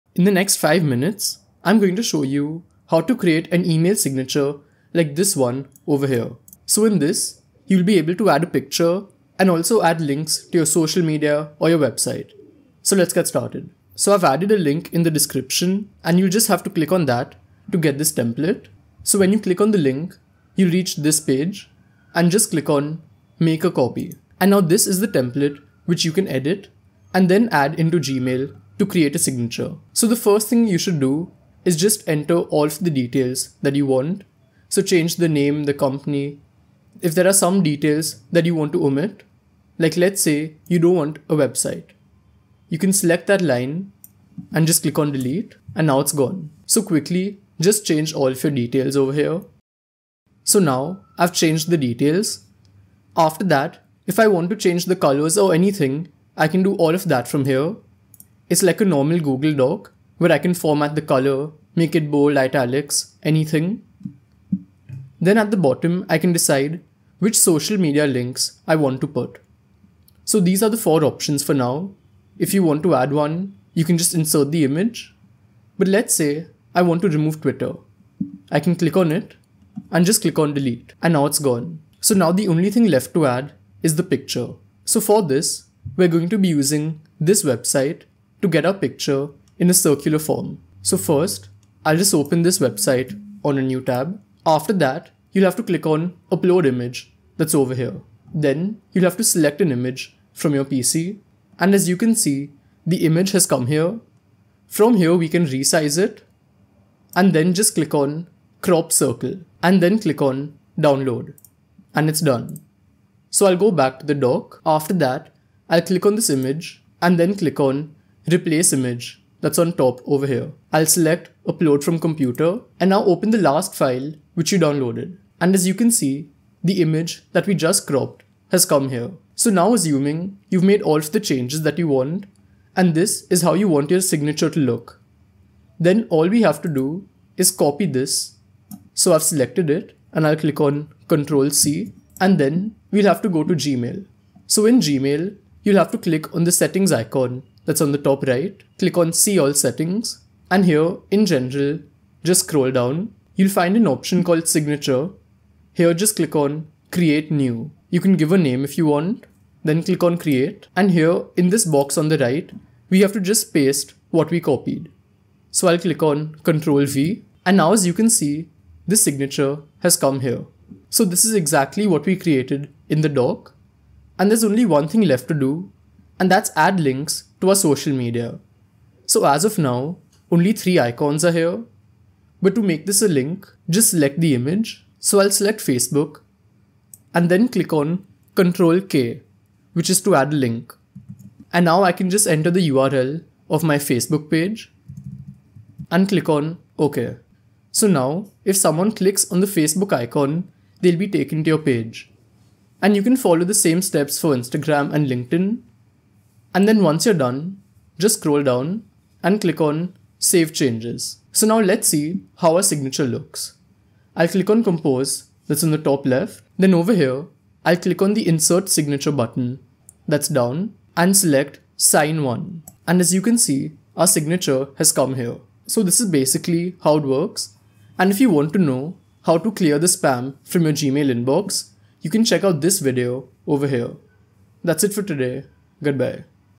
How To Add Signature in Gmail (With Professional Design)
Make your emails uniquely yours! In this tutorial, we'll guide you through the process of creating a standout Gmail signature that includes a personalized picture and links to your social media profiles. Elevate your email game and leave a lasting impression with every message you send.

In this video, we'll cover:
Accessing Gmail Settings: Learn how to navigate to your Gmail settings where you can find the signature customization options.
Creating Your Signature: We'll guide you through the steps of crafting a signature that reflects your personal or professional style.
Adding a Picture: Discover how to upload and incorporate a profile picture or company logo into your Gmail signature.
Inserting Social Media Links: Elevate your email signature with links to your social media profiles. We'll show you the easy steps to include clickable icons for a professional touch.

By the end of this tutorial, you'll have a polished and personalized Gmail signature that sets you apart.